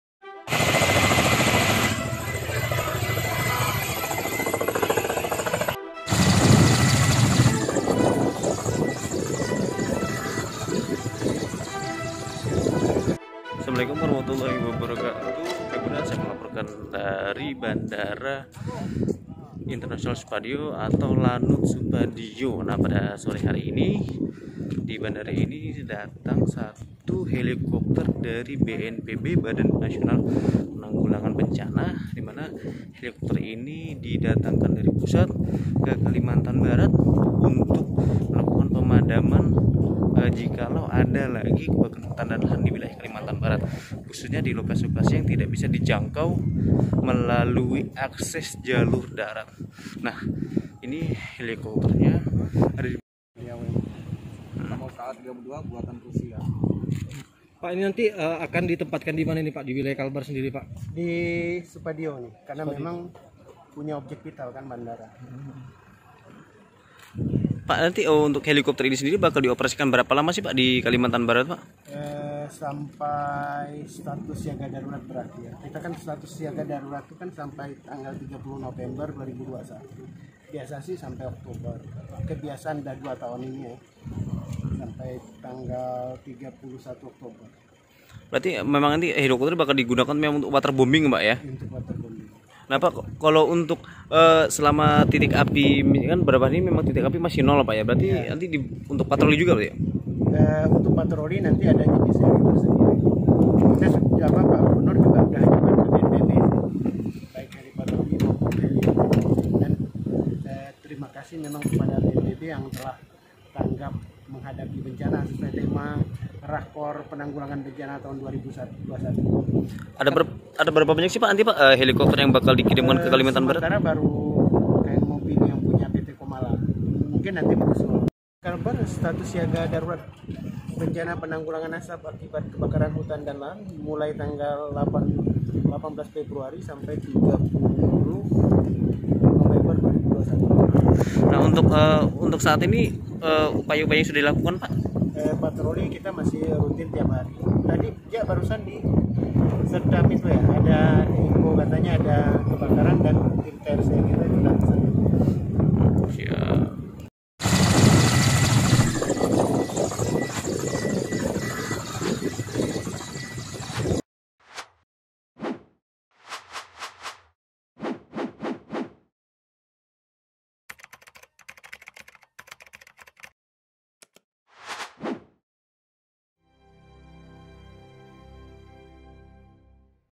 BNPB Kirim Bantuan Helikopter untuk Penanganan Kebakaran Lahan di Kalbar
BNPB (Badan Nasional Penanggulangan Bencana) mengirimkan satu unit helikopter ke Kalbar (Kalimantan Barat) guna Proses penanggulangan kebakaran Lahan di Kalbar.

Helikopter Jenis Kamov KA 32 C tersebut datang ke Kalbar pada Jumat 6 Maret 2021 sekira pukul 16.45 WIB dan langsung mendarat di Lanud Supadio.

Serah terima Helikopter yang bakal digunakan untuk Waterbombing ke lokasi lahan terbakar itu dipimpin langsung oleh Kepala BPBD Provinsi Kalbar Lumano.

Lumano mengatakan bahwa beberapa waktu lalu Gubernur Kalbar sudah mengirimkan surat ke BNPB untuk proses permohonan bantuan Helikopter guna Waterboombing dan TMC (Teknologi Modifikasi Cuaca), dan saat ini bantuan Helikopter telah disetujui.

Nantinya Helikopter buatan Rusia ini akan bersiaga di Kalbar hingga status siaga darurat bencana kebakaran lahan di Kalbar berakhir, Siaga Darurat Karhutla di Kalbar yakni sejak 18 Februari hingga30 November 2021.

Helikopter ini akan betugas melakukan pemadaman kebakaran lahan di titik - titik yang tidak dijangkau Pemadam melalui jalur darat.

"untuk sementara satu unit bantuan Helikopter untuk Waterbombing, dan nanti bisa menyusul lagi melihat situasi dan kondisi di Kalbar,"ujarnya.

Terkait TMC dan Waterboombing dikatakan Lumano merupkan kebijakan penuh dari BNPB, daerah hanya mengajukan permohonan.

Kemudian, terkait Operasional Waterboombing maupun TMC yang dikenal cukup tinggi, Lumano menjelaskan seluruhnya akan ditanggung oleh Pusat.